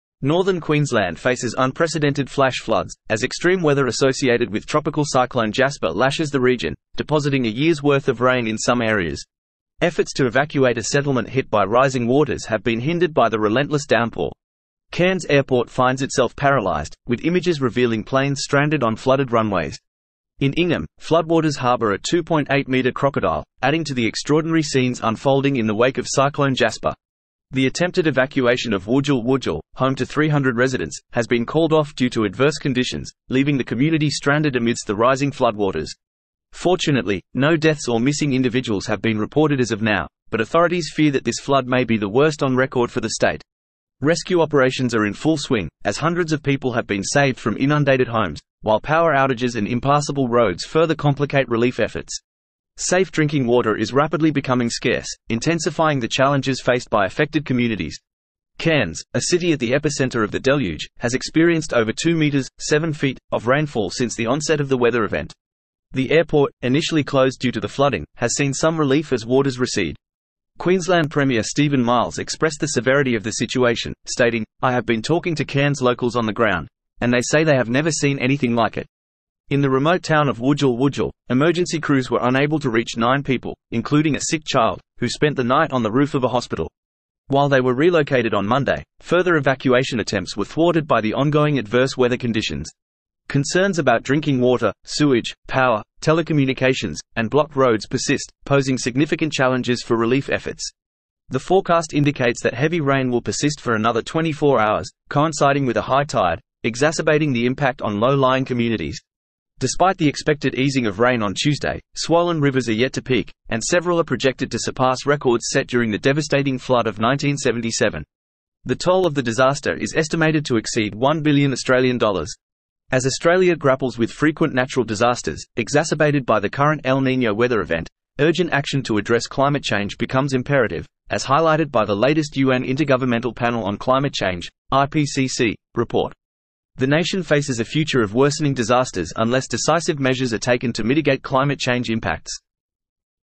Flash Floods Devastate Northern Queensland as Cyclone Jasper Unleashes Year's Worth of Rainfall
Flash floods have inundated parts of northern Queensland, Australia, with the heavy rain thwarting attempts to evacuate a settlement hit by rising water.